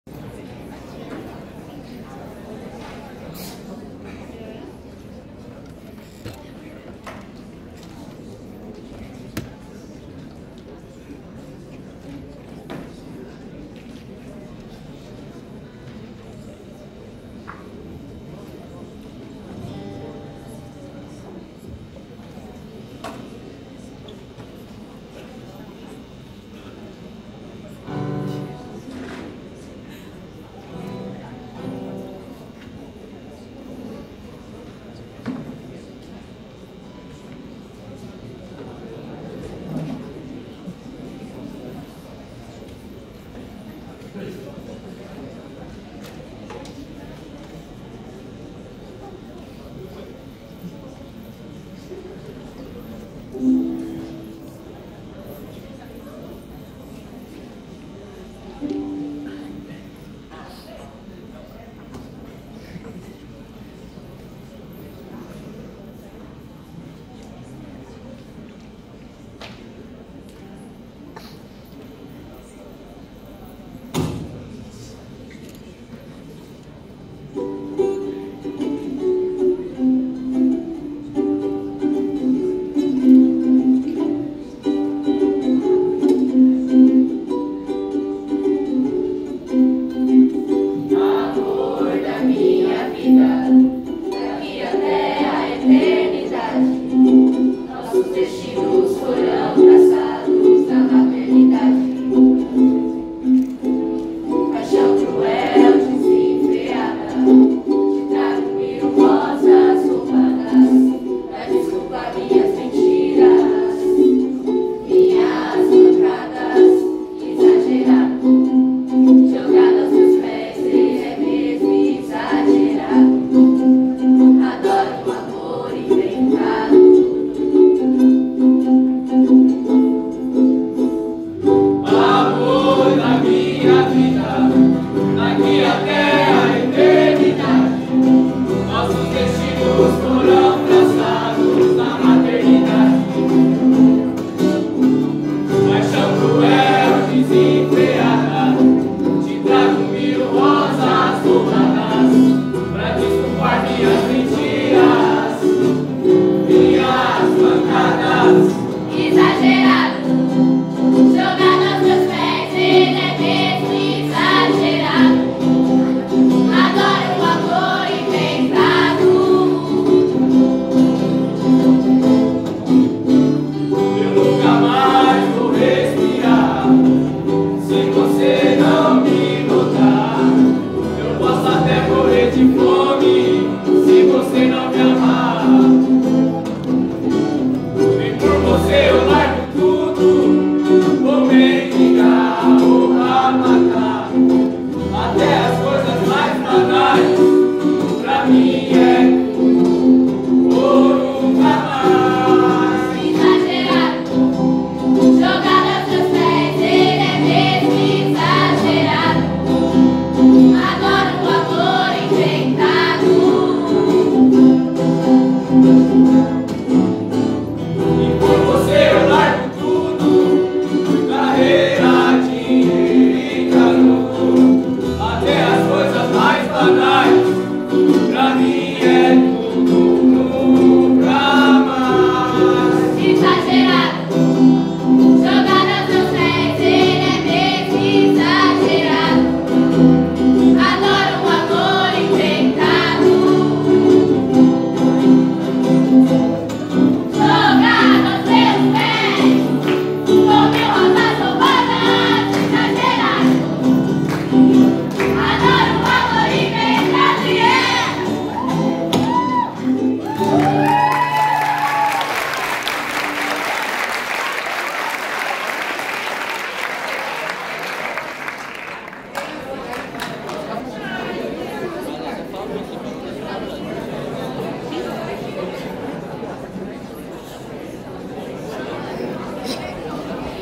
Sarau Literário 2018 Francisco Ferreira Lopes 29
No Teatro Vasques de Mogi das Cruzes ocorreu em 04 de Setembro de 2018 a apresentação do Projeto Sarau Literário - Séc XIX e XX: Diálogos e Intertextos com alunos da Escola Estadual
Francisco Ferreira Lopes do período da manhã e tarde e com a idealização e coordenação dos professores Anderson Zaneti, Sandra Horita e Ana Cristina.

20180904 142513